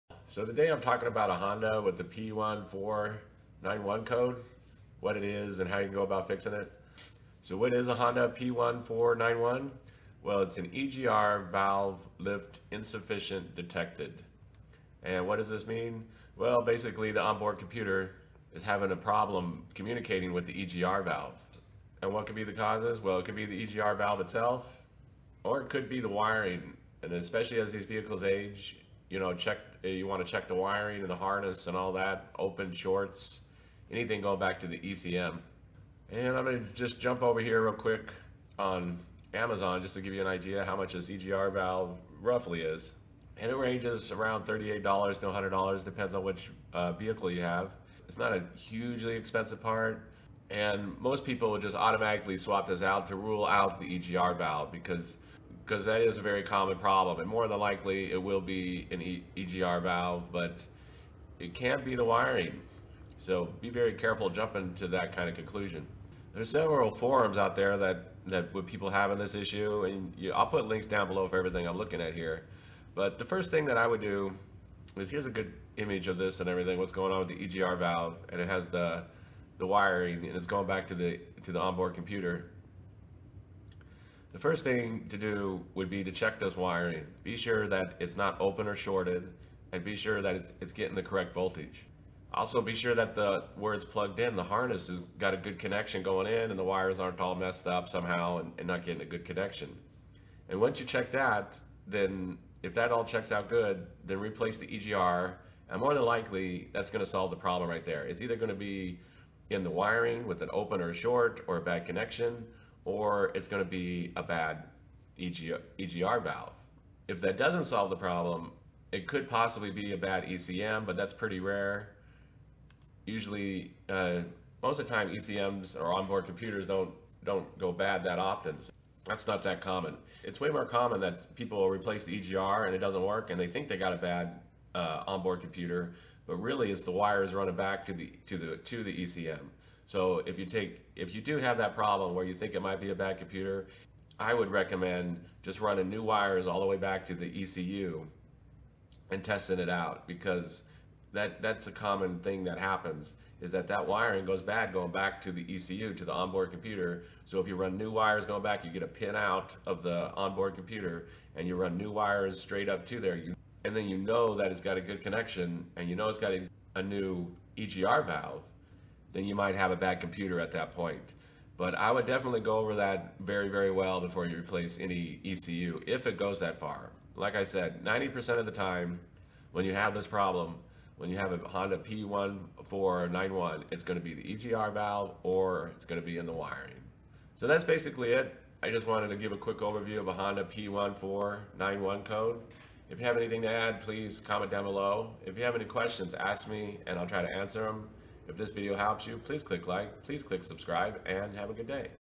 How To Fix Honda P1491: EGR Valve Lift Insufficient Detected
Overview of the Causes and Fixes of a Honda P1491 OBDII error code: EGR Valve Lift Insufficient Detected
Causes: EGR Valve or Wiring